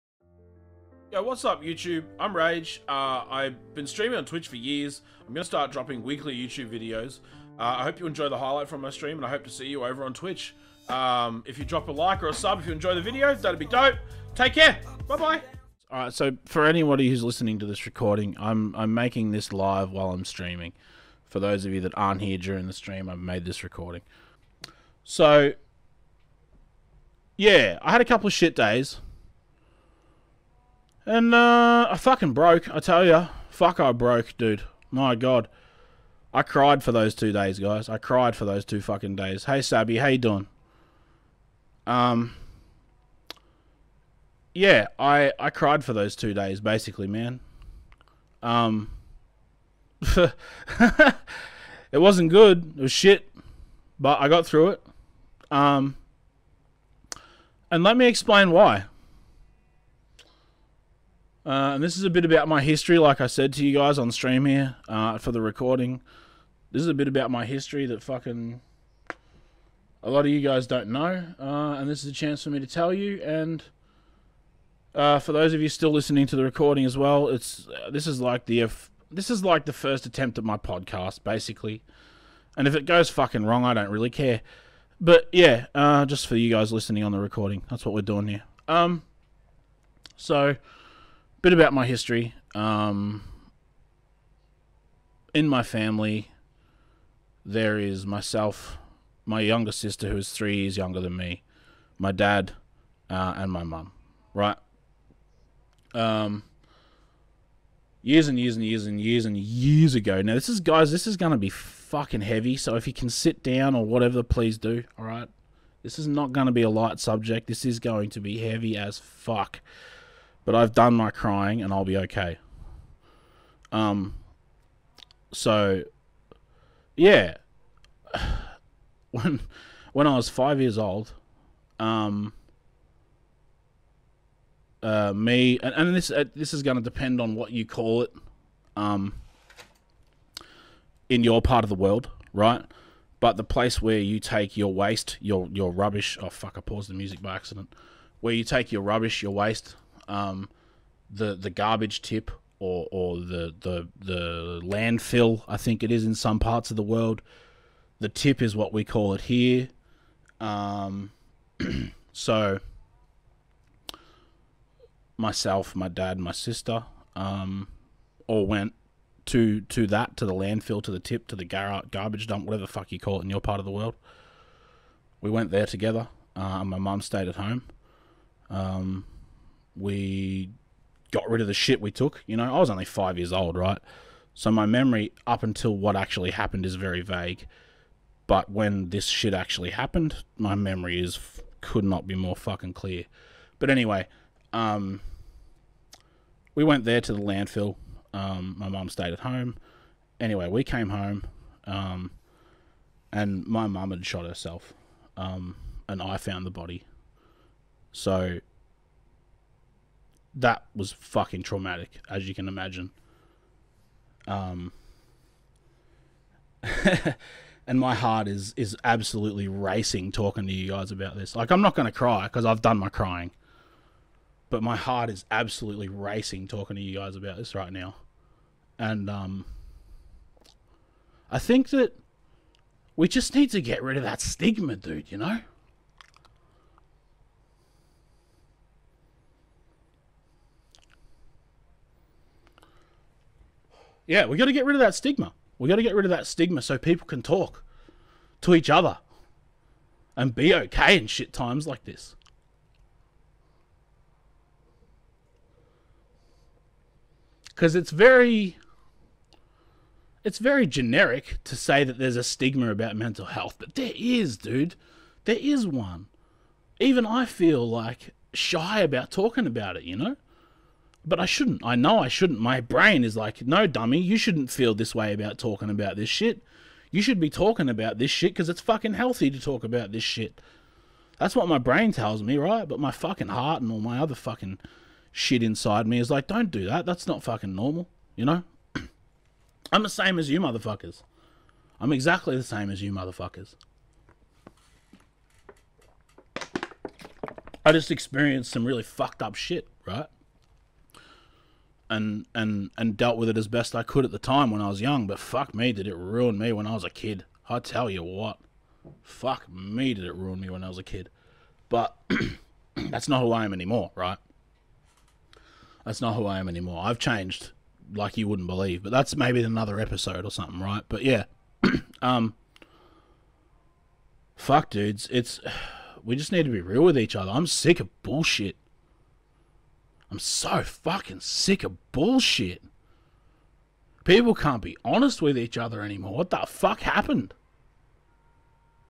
Some Thoughts About Mental Health.
In this recording I share some of my life experience from one of my streams over the last 4 years on twitch, Mental Health is important to speak about to people in your life.

We all have life experiences and sharing them can help us grow. Big love nerds!